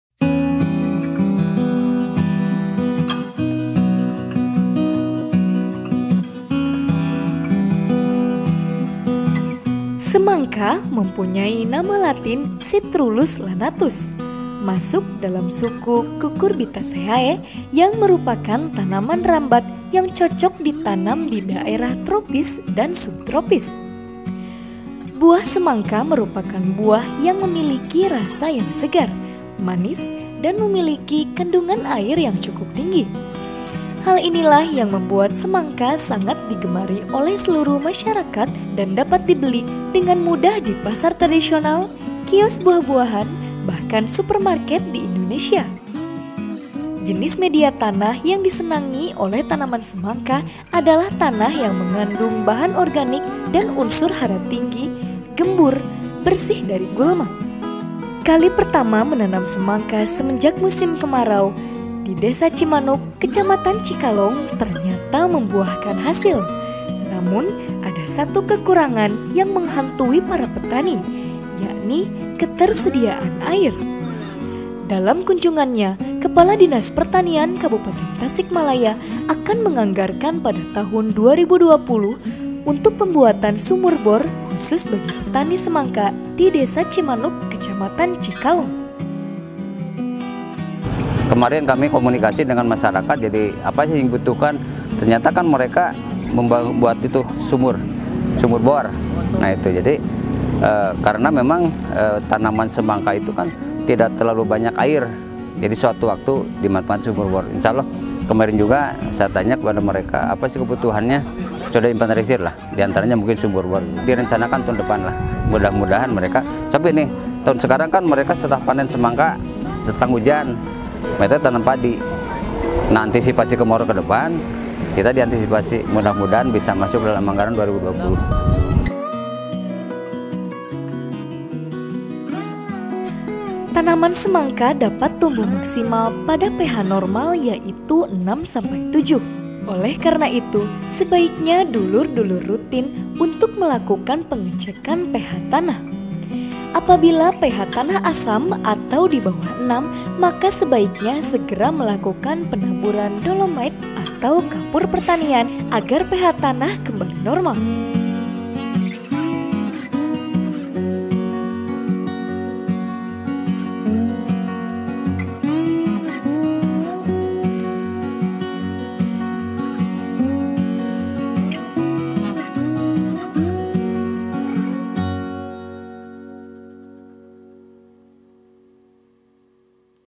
ANGIN SEGAR, PETANI DI CIKALONG AKAN DI ANGGARKAN SUMUR BOR PADA TAHUN 2020
Semangka mempunyai nama Latin Citrullus lanatus, masuk dalam suku Cucurbitaceae yang merupakan tanaman rambat yang cocok ditanam di daerah tropis dan subtropis.

Buah semangka merupakan buah yang memiliki rasa yang segar, manis dan memiliki kandungan air cukup tinggi. Hal inilah yang membuat semangka sangat di gemari oleh seluruh masyarakat dan dapat dibeli dengan mudah di pasar tradisional, kios buah bahkan supermarket di Indonesia.

Jenis media tanah yang disenangi oleh tanaman semangka adalah tanah yang mengandung bahan organik dan unsur hara tinggi, gembur, bersih dari gulma.

kali pertama menanam semangka semenjak musim kemarau ternyata membuahkan hasil, Di kecamatan cikalong desa cimanuk ternyata membuahkan hasil, namun ada satu kekurangan yang menghantui para petani, yakni ketersediaan air, dalam kunjungannya, kepala dinas pertanian kabupaten tasikmalaya akan menganggarkan pada tahun 2020 untuk pembuatan sumur bor khusus bagi petani semangka di desa ci manuk kecamatan cikalong.

Tanaman semangka dapat tumbuh maksimal pada pH normal yaitu 6-7. Oleh karena itu, sebaiknya dulur-dulur rutin untuk melakukan pengecekan pH tanah. Apabila pH tanah asam atau dibawah 6, maka sebaiknya segera melakukan penaburan dolomite atau kapur pertanian agar pH tanah kembali normal.